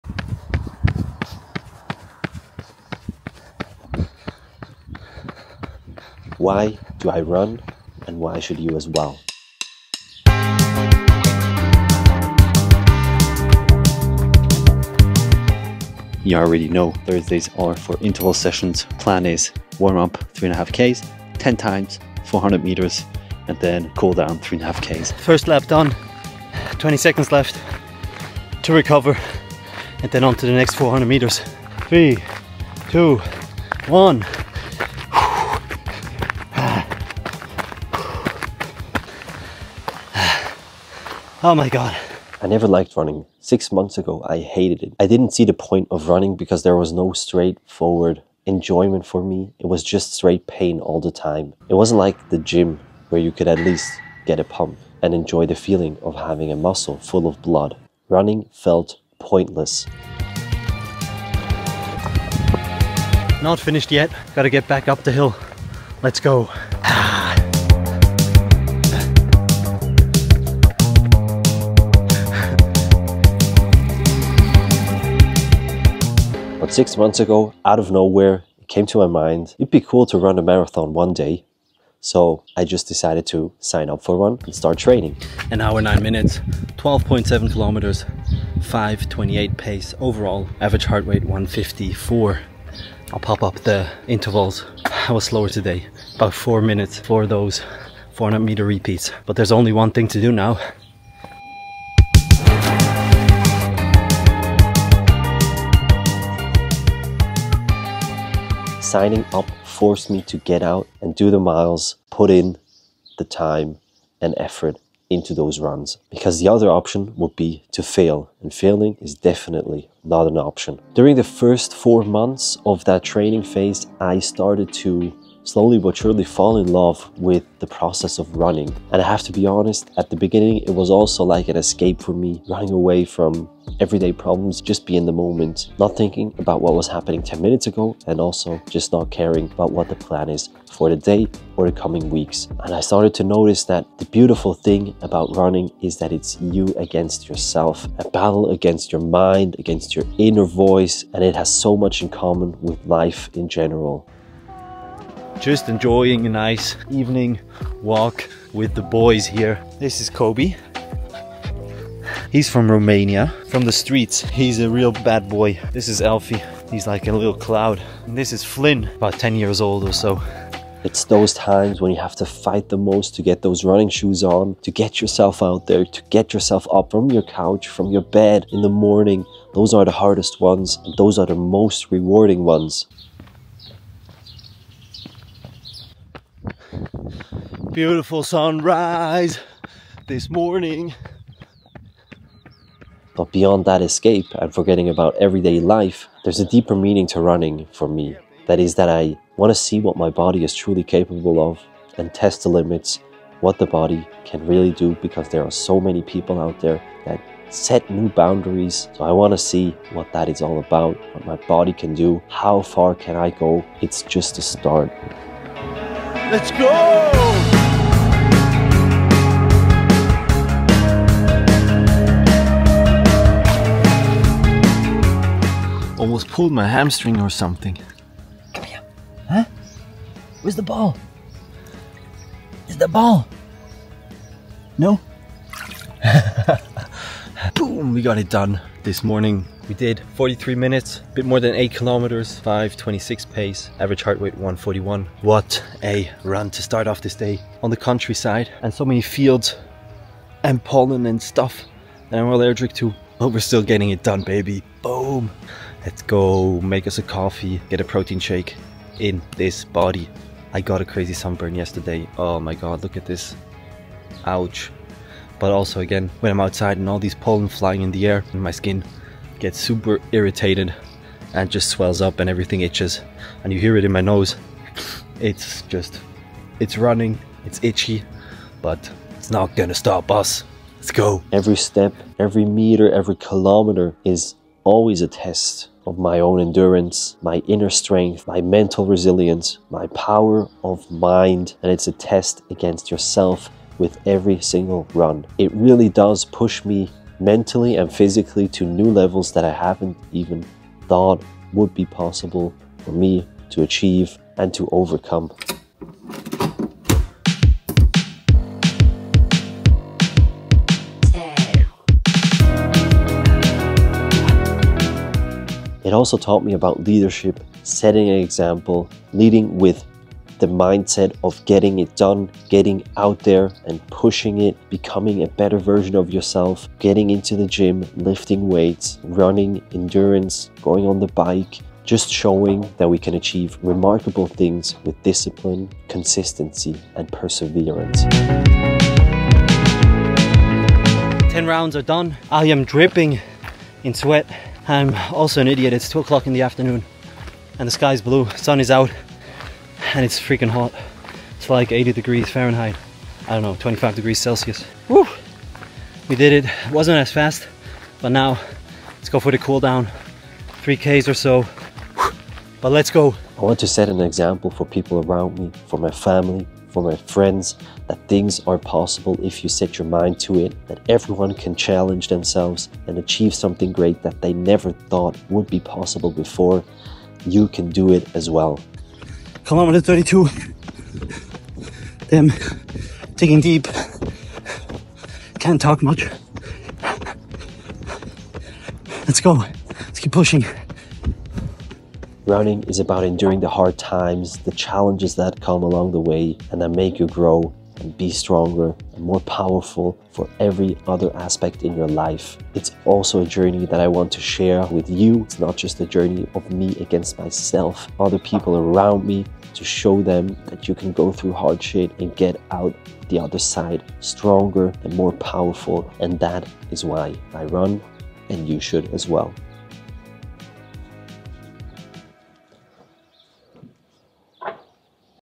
WHY I RUN (and you should too)
Discover why I run and why you should too. Running has transformed my life, helping me overcome mental barriers and push my physical limits. In this video, I share my journey, the lessons I've learned, and the incredible rewards of embracing running. Plus, get a glimpse into my training routine with interval sessions and workouts.